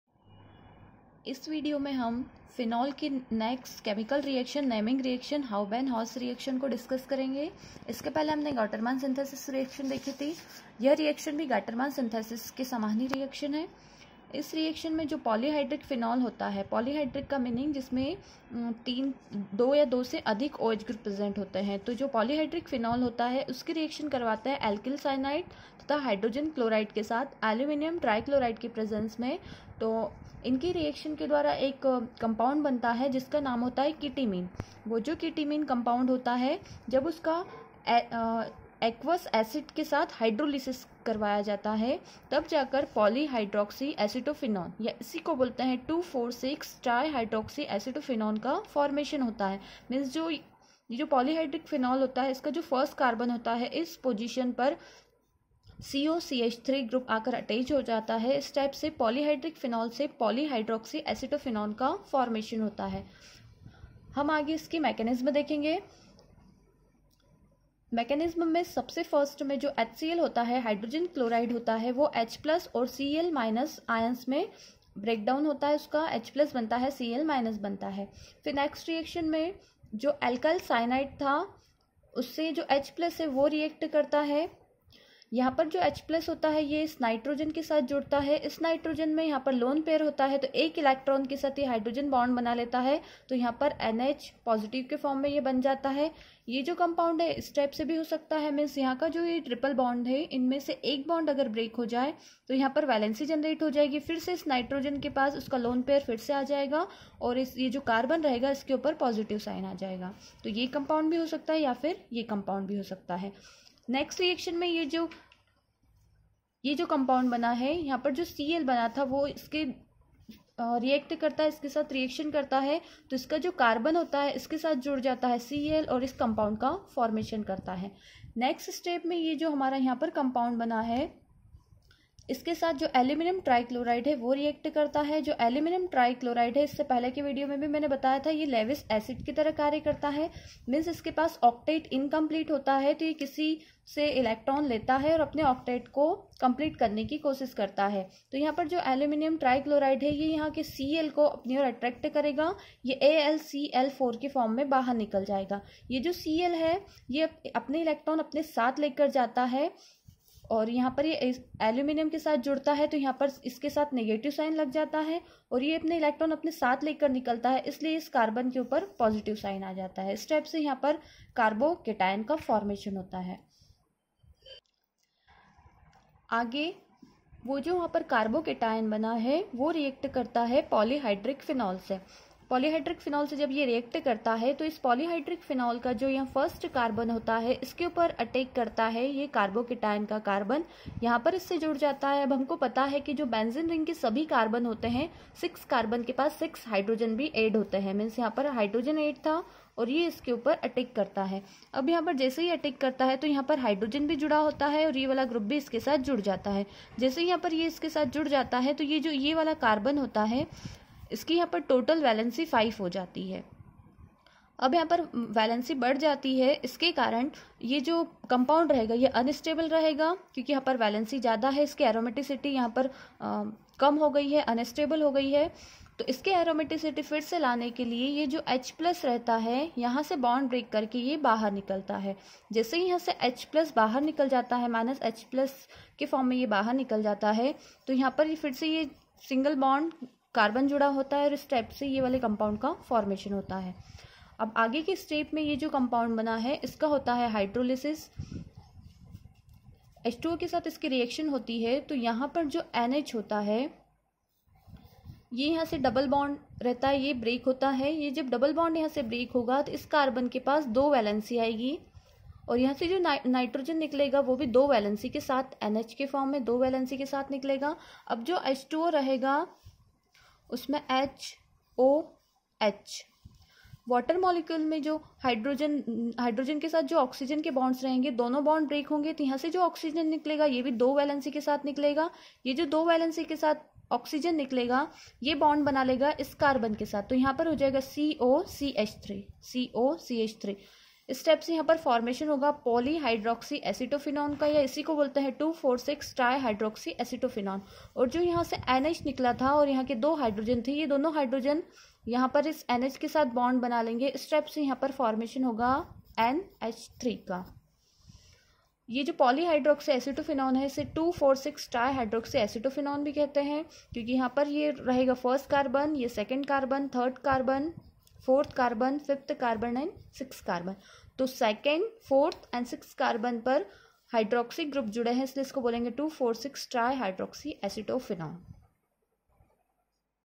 Lecture-6.5: Phenols- Hauben- Hoesch Reaction by Miss Sapna Chouhan
B.Sc II year Organic chemistry (Unit II)